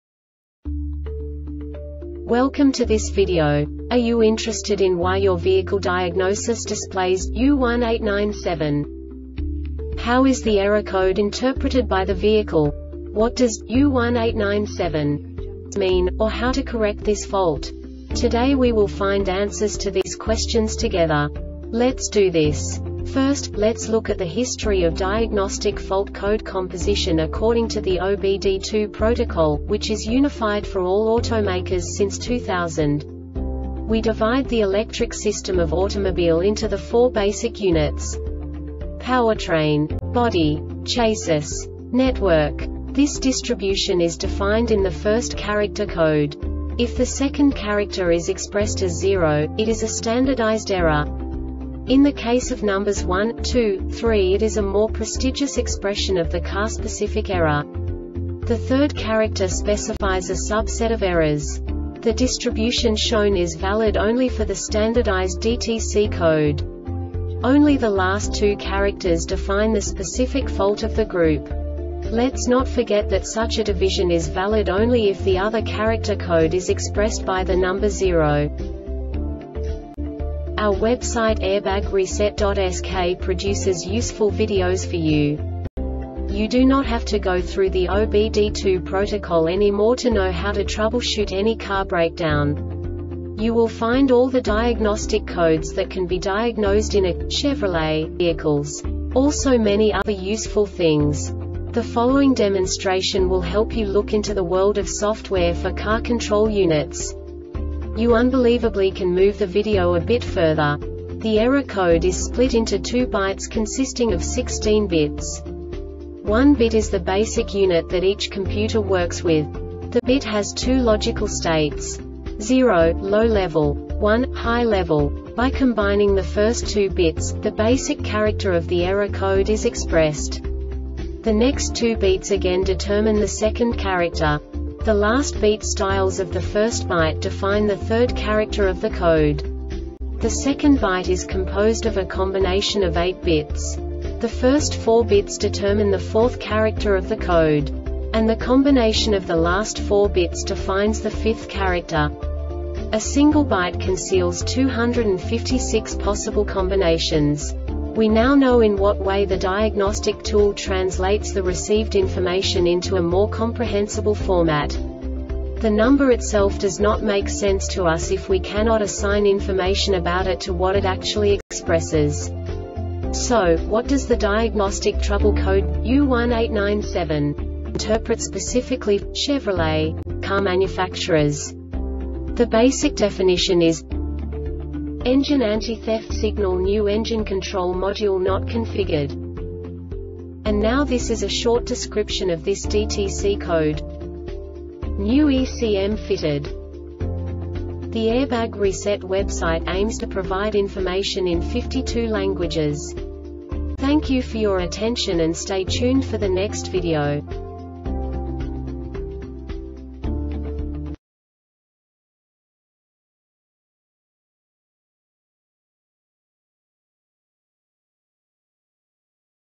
DTC Chevrolet U1897 Short Explanation
Contents:
0:21 Basic DTC analysis according to OBD2 protocol standard.
1:48 Insight into programming
2:58 A diagnostically understandable description of the Diagnostic Trouble Code
3:05 Basic error definition

Make:
Chevrolet
Code:
U1897
Definition:
Engine anti-theft signal new engine control module not configured
Description:
New ECM fitted